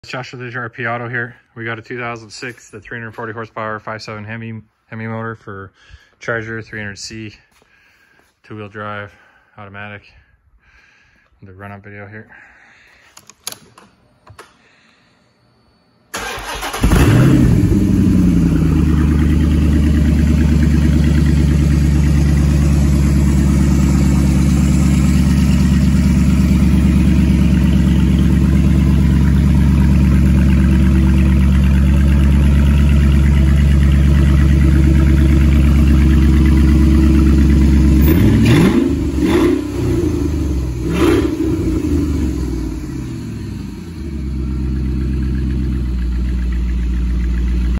340HP 5.7 Hemi Running Pallet. 2wd auto, Charger/ 300c **Sold to Dubs, Rust Valley** SWAP!
Turn key 2wd 5.7 hemi (2006/2007). running on the skid. Comes with everything you see including front subframe and steering rack. Had approx 130k miles (pulled from US Spec car).